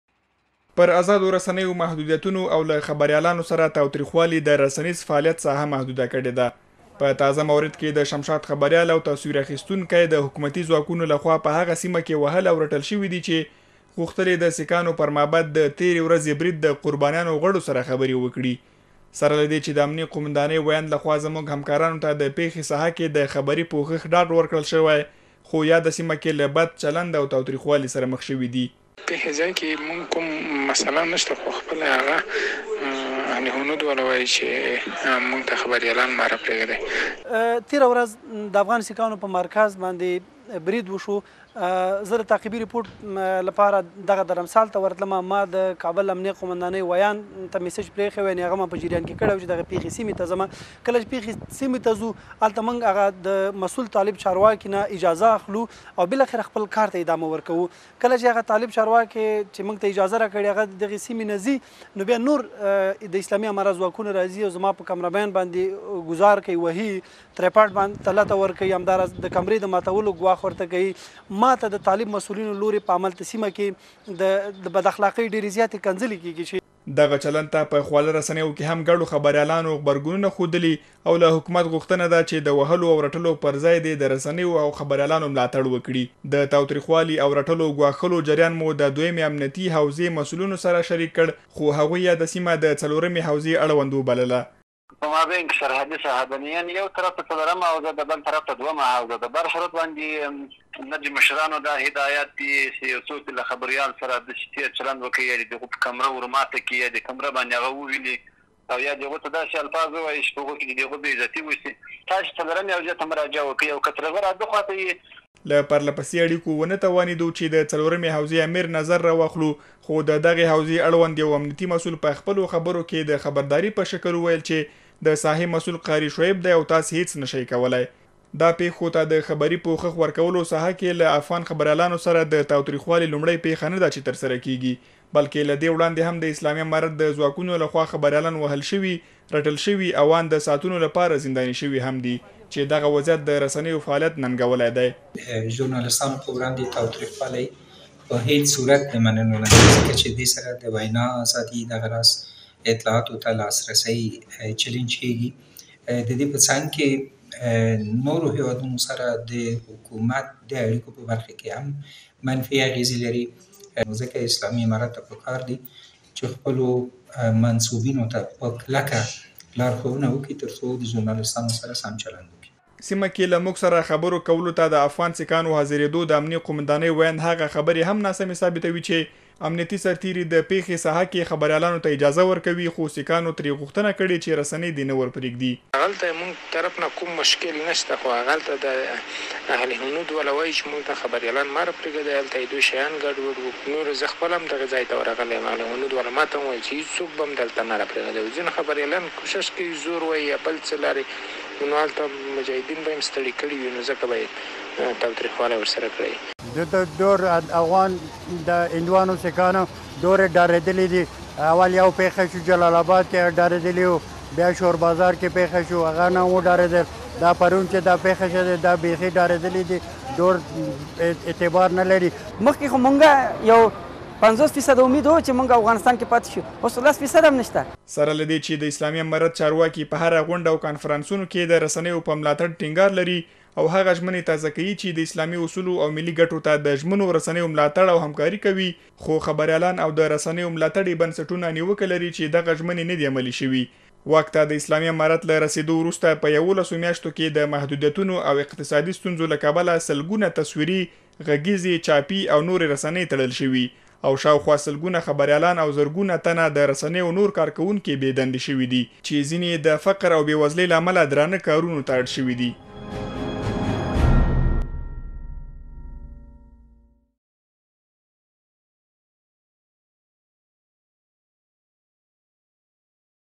خبریالانو سره تاوتریخوالي د رسنیز فعالیت ساحه محدوده کړې ده.
په تازه مورد کې امنیتي ځواکونو په کابل کې د شمشاد خبریال او تصویر اخیستونکي سره  د تیرې ورځې پيښې سیمه کې تاوتریخوالی کړی.
دغه چلند خبریالان او د رسنیو ملاتړي بنسټونه اندېښمن کړي او له حکومت غواړي چې پر رسنیو د محدودیتونو لګولو پرځای دې د مطبوعاتو ملاتړ وکړي.